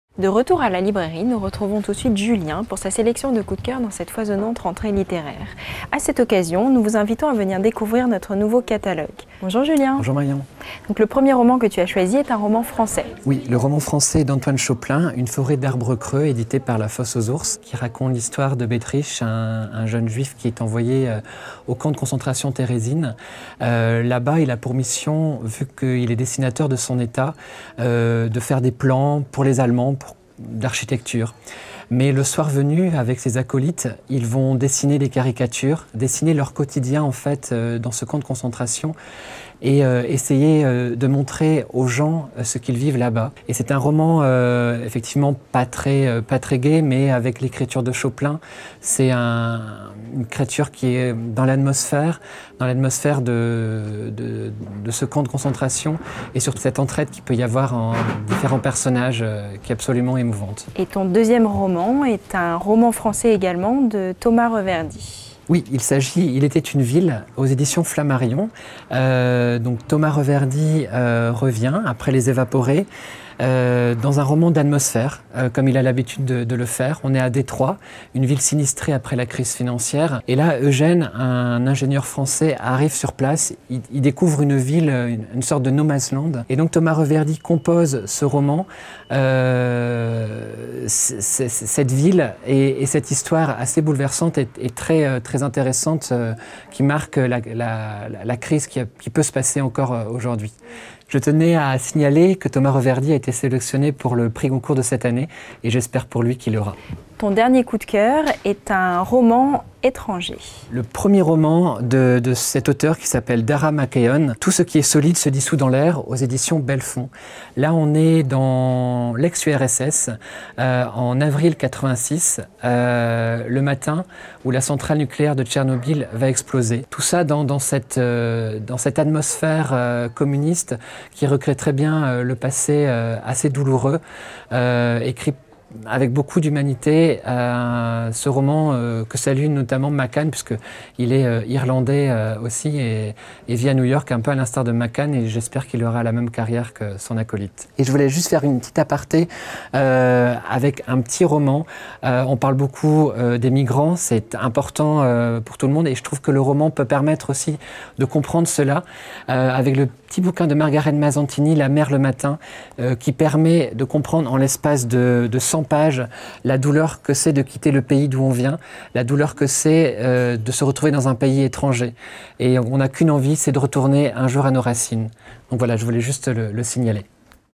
La chronique rentrée littéraire de Julien, Dialogues littéraires, septembre 2015
Julien de la librairie Dialogues nous propose ses coups de cœur de la rentrée littéraire 2015 : Une forêt d'arbres creux d'Antoine Choplin (La fosse aux ours), Il était une ville de Thomas B. Reverdy (Flammarion), Tout ce qui est solide se dissout dans l'air de Darragh Mc Keon (Belfond) et La mer, le matin de Margaret Mazzantini (10-18).
Réalisation : Ronan Loup.
Questions posées par : Marion Le Goascoz.